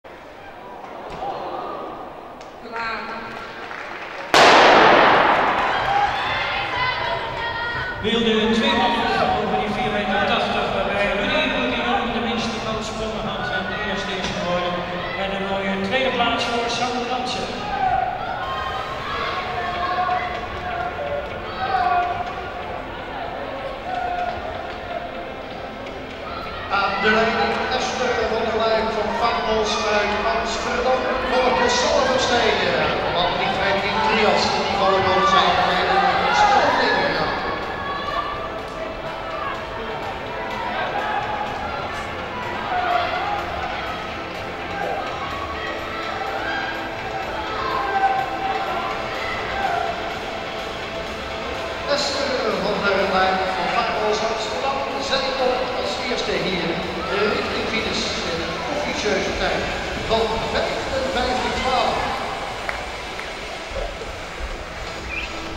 400m Vrouwen serie 6
In deze voorlaatste 400m serie loopt Esther van der Lycke naar de winst in een nieuw persoonlijk record van 55.11. Sanne Verstegen en Graciëlla Woutersen vechten om de 2e plek en finishen resp. als 2e en 3e in 56.45 om 56.46.